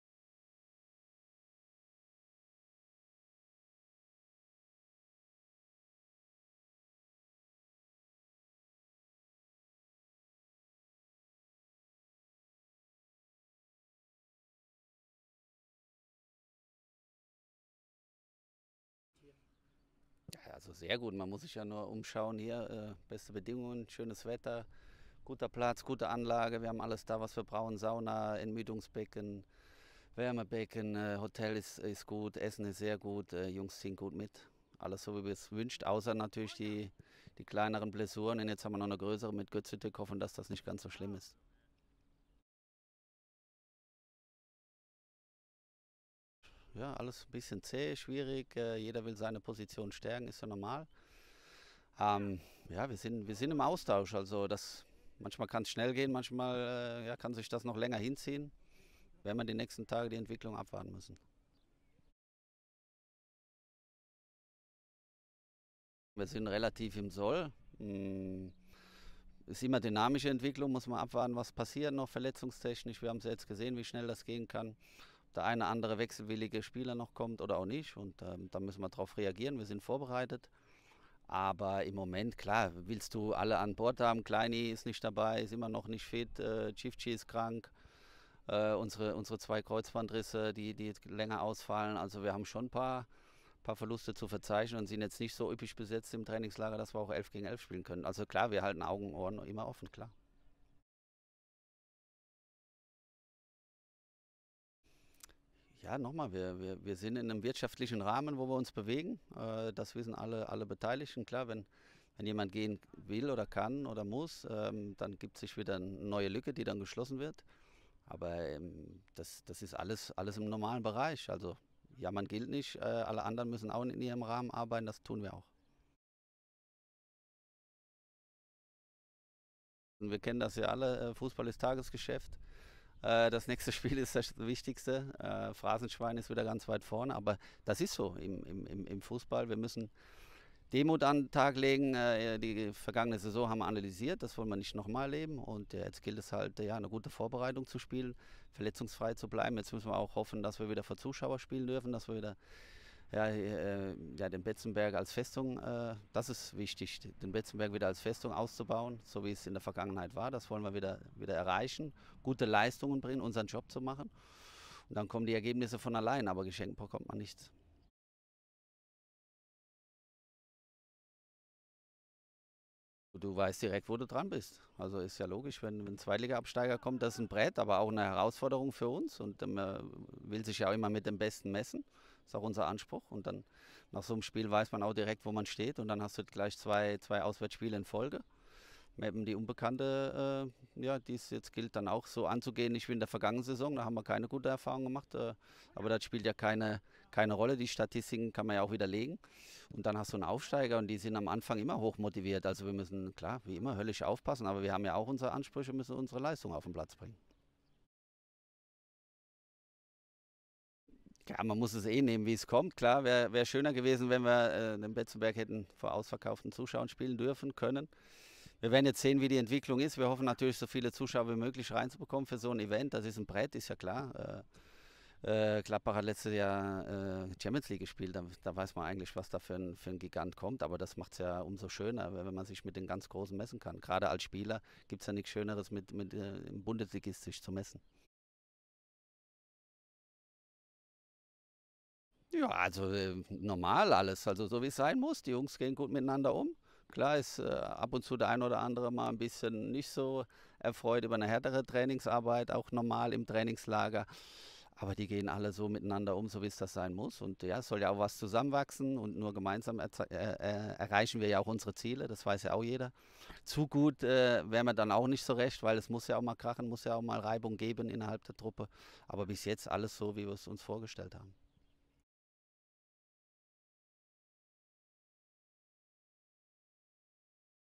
FCK: Sport-Geschäftsführer Thomas Hengen im RHEINPFALZ-Gespräch
Er hat auch in Südtirol einiges zu tun, bastelt weiter am Kader für die am 24. Juli gegen Eintracht Braunschweig startende Drittliga-Saison. Im Gespräch mit der RHEINPFALZ zeigt sich Thomas Hengen, der Sport-Geschäftsführer des 1. FC Kaiserslautern, angetan von den Bedingungen in Mals. Video: Sebastian Stollhof